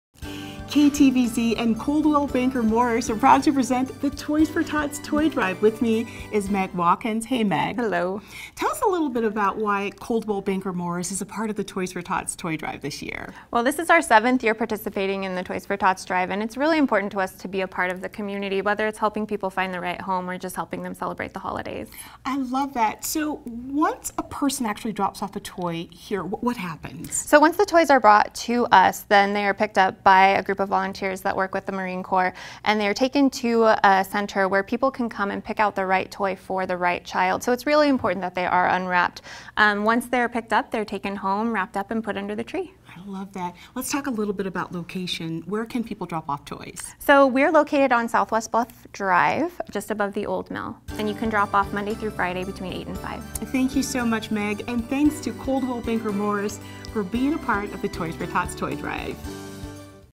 For KTVZ, Tracee Tuesday's with Meg from Coldwell Banker supporting the Toys For Tots Program!
For KTVZ, Tracee Tuesday's with Meg from Coldwell Banker, supporting the Toys For Tots Program 2017!